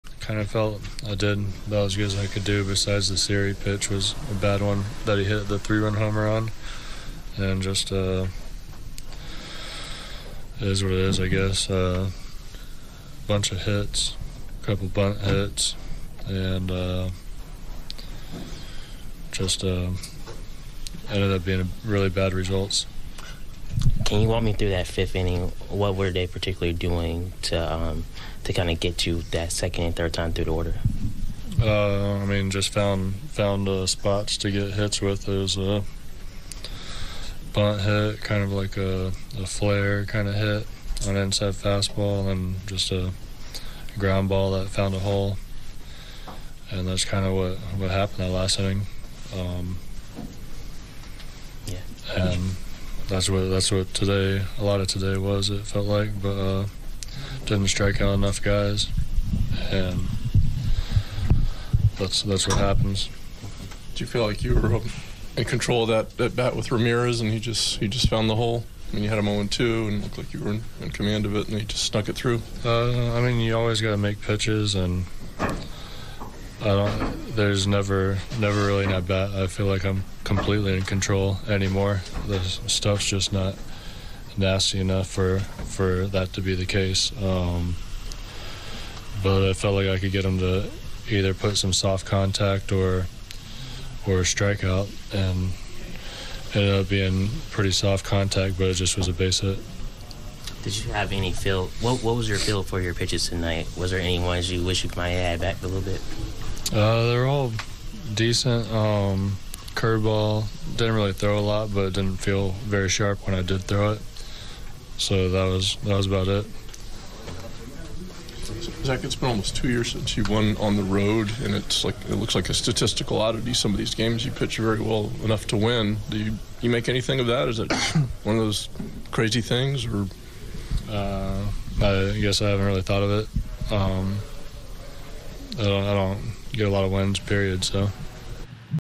Greinke: 'There's never really an at-bat I feel like I'm completely in control anymore'
Zack Greinke: "I didn't strike out enough guys, and that's what happens. You always have to make pitches, and there's never really an at-bat I feel like I'm completely in control anymore. The stuff's just not nasty enough for that to be the case."